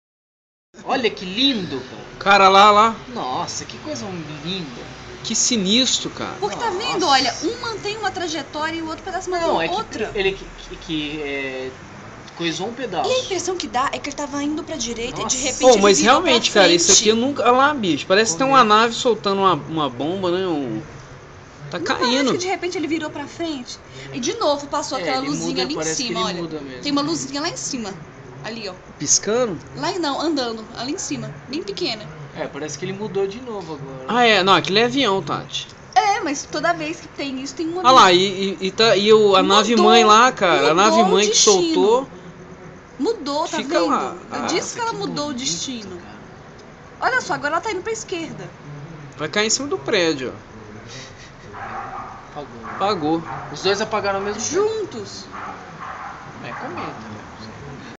OVNI, UFO ou Meteoros? Brasilia (Video 2)
Imagens captadas da ASA NORTE de BRASILIA às 18h24m32s do dia 03 de Junlho de 2011. O evento ocorreu nos céus da saída sul de Brasilia.
Este é video 2.